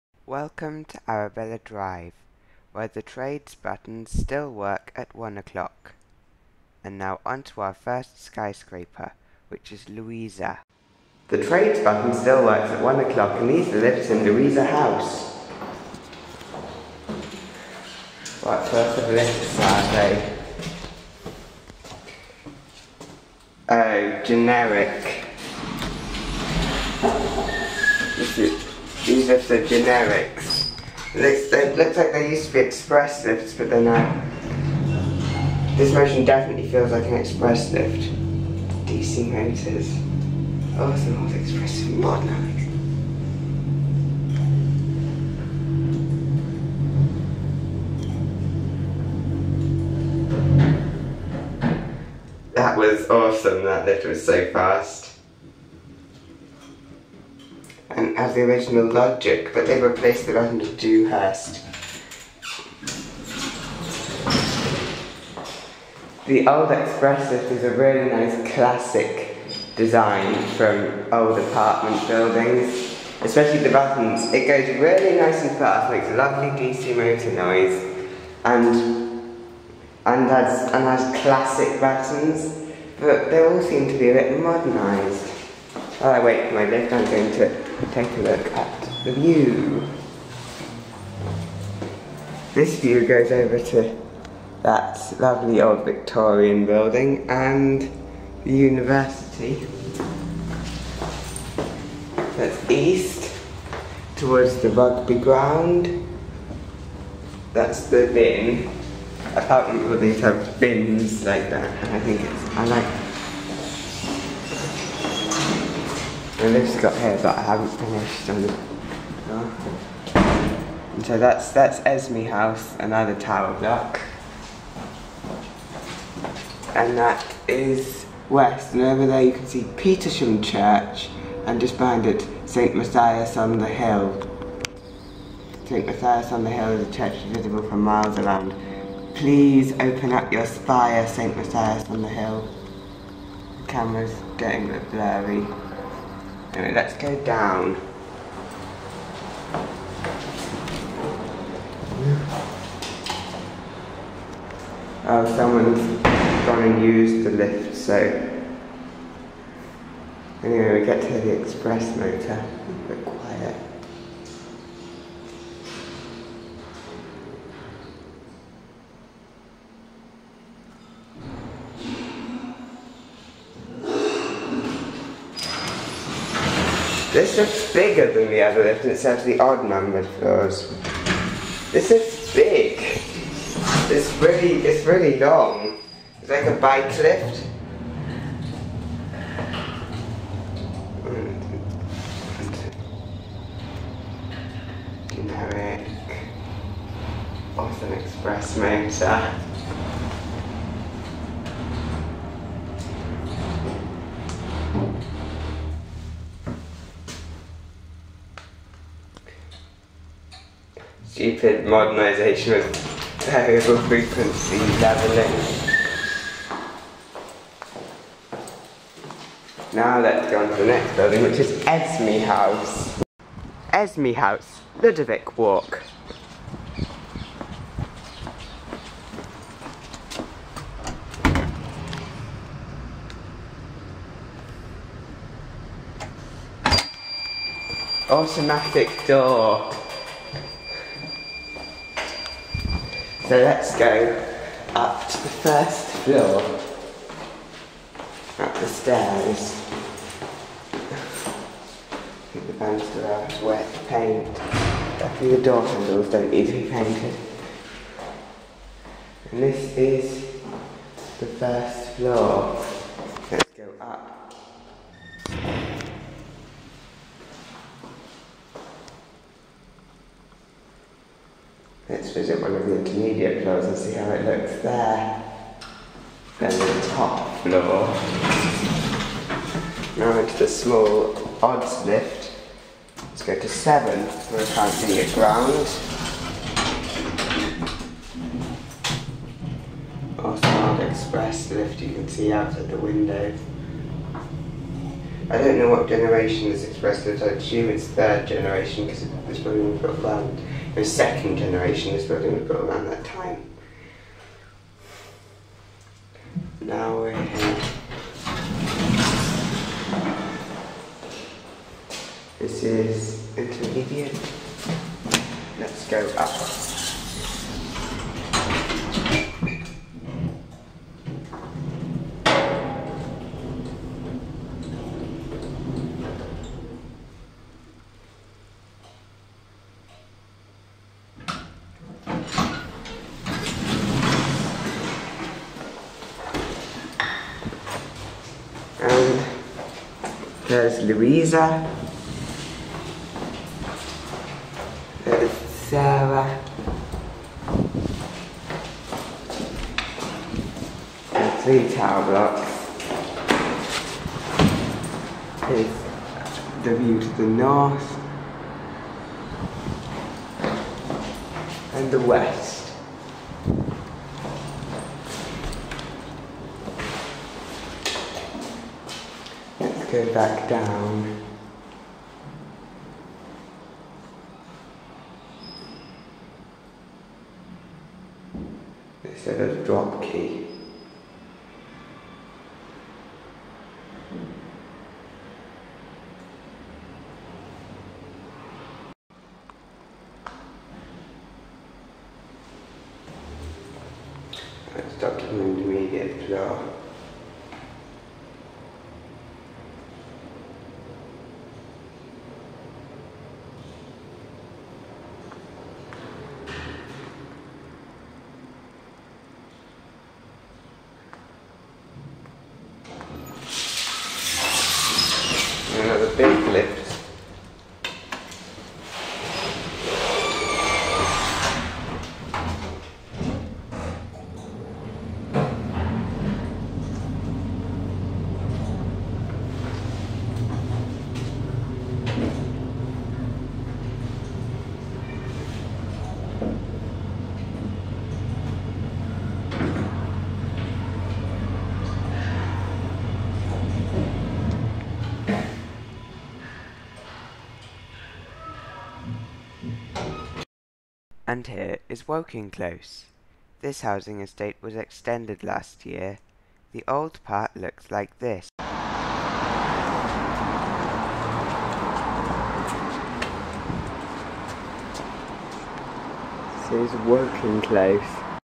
Louisa & Esme
These are 2 tower blocks in a set of 3 (I didn't go in the third to save time & because it would probably be identical to one of the ones I'd already been in). The lifts here are arranged in with one for evens, one for odds. The lift on the left is bigger because the lift on the right has to fit the bin behind it. They are fast old lifts (Maybe Express or Bennie) that have been modernised. The logic is original, but the buttons & interior have been replaced. The motor is a bit strange, because it sounds like a DC motor & the lifts start off & travel most of their journey at a single fast speed, but when they get to a floor, they level very slowly.
|10|10|       | 9 | 9 |
| 9 | -- |        | 8 | -- |
| -- | 8 |        | -- | 7 |
| 7 | -- |        | 6 | -- |
| -- | 6 |        | -- | 5 |
| 5 | -- |        | 4 | -- |
| -- | 4 |        | -- | 3 |
| 3 | -- |        | 2 | -- |
| -- | 2 |        | -- | 1 |
| 1 | -- |        | G | G |
| G | G |       |LG|LG|